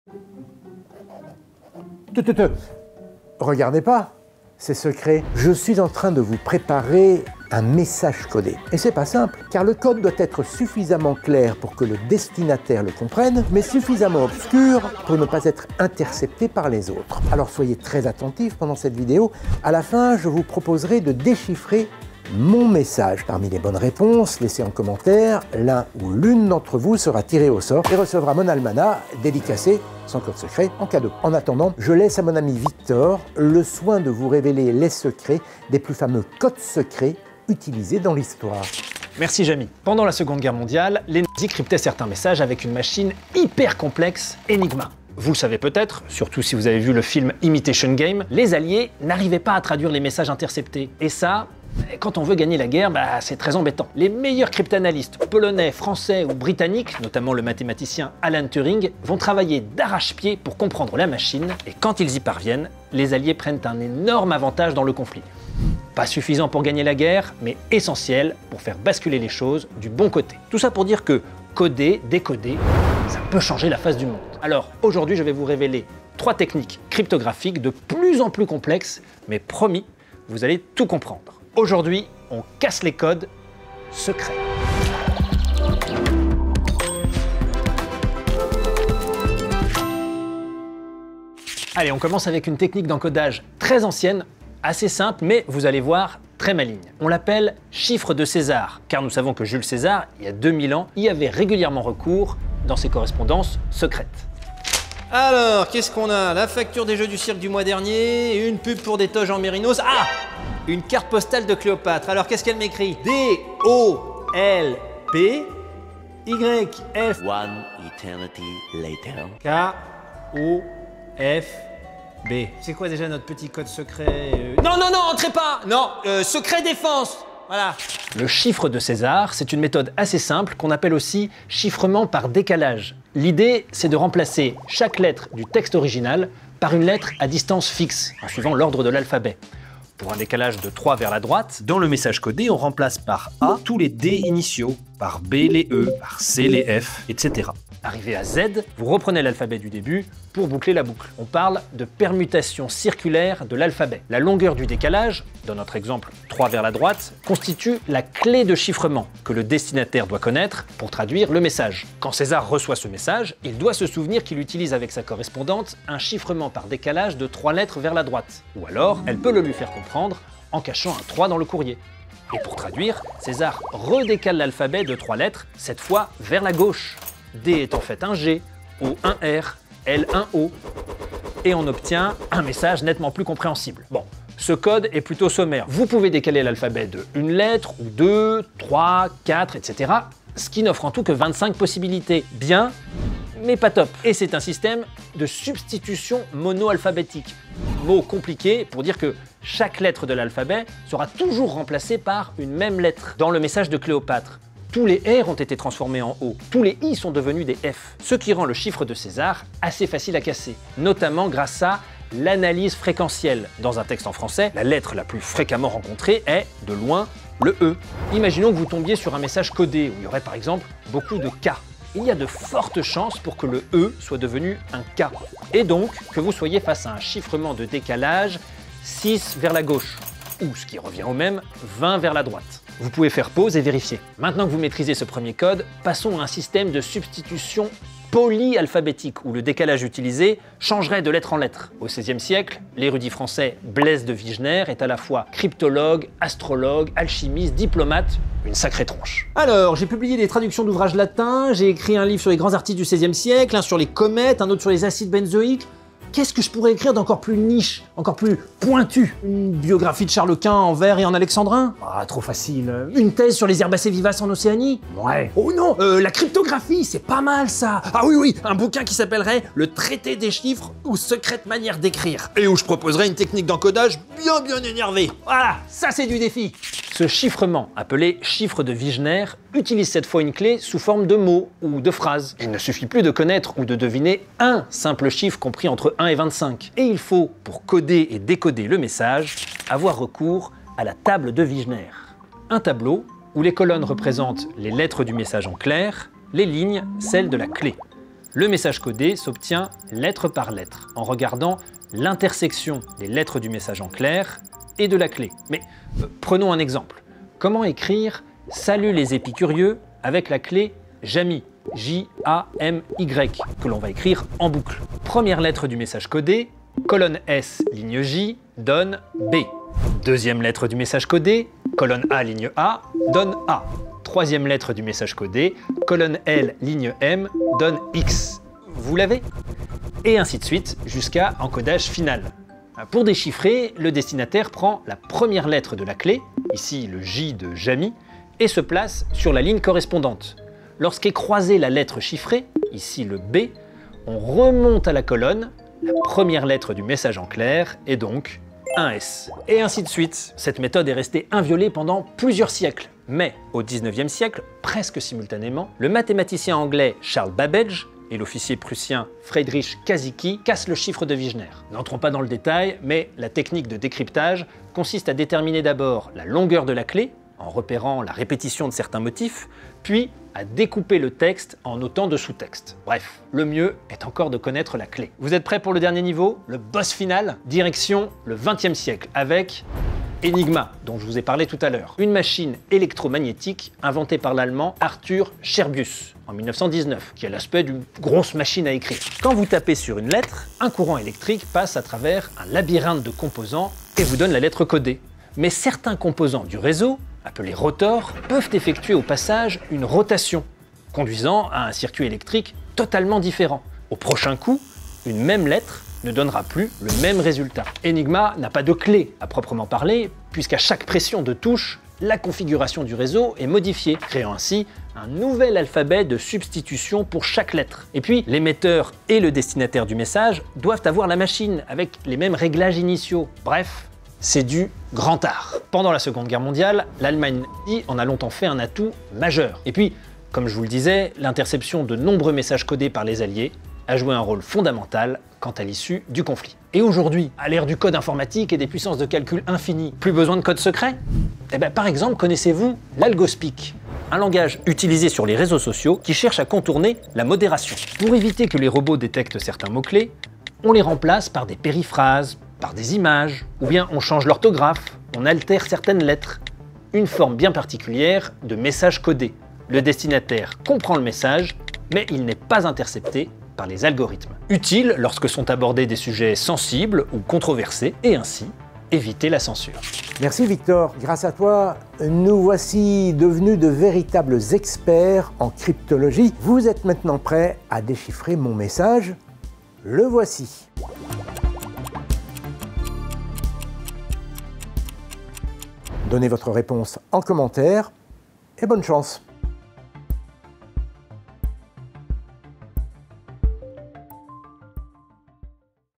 Comment déchiffrer un code secret ? 🤫
Les codes secrets ont façonné l’Histoire et continuent de nous entourer… Mais savez-vous vraiment comment ils fonctionnent ? 🔐

Derrière les lettres chiffrées et les algorithmes sophistiqués se cachent des techniques ingénieuses qui ont protégé des secrets, influencé des guerres, et même modifié notre façon de communiquer aujourd’hui.

Du chiffre de César aux langages codés d’Internet, en passant par la célèbre machine Enigma, découvrez comment l’art du cryptage a évolué au fil du temps.

Comment fonctionnent ces systèmes de codage ?
Pourquoi certaines méthodes sont-elles si difficiles à décrypter ?
Et surtout… Saurez-vous résoudre l’énigme que Jamy vous a préparée ? (parmi ceux qui y parviendront en commentaires, une personne sera tirée au sort pour remporter le dernier livre dédicacé de Jamy ! 📕)

Les Épicurieux ont déchiffré pour vous les mystères de la cryptographie ! 🕵️‍♂️

Merci à Victor (@victorpiersoninsta) pour sa contribution ! 🙏 (Jamy va très bien 😉)

0:00 Intro
1:31 Le chiffre de César
3:54 Le chiffre de Vigenère
6:15 Enigma, la machine n@zie
7:16 L’algospeak
7:50 Trouvez le code de Jamy !
08:06 Conclusion

Et Victor :
Youtube ➡️    / @victorpiersonyoutube
Instagram ➡️   / victorpiersoninsta
TikTok ➡️   / victorpiersontiktok

Montage : Pratheep Sivasiri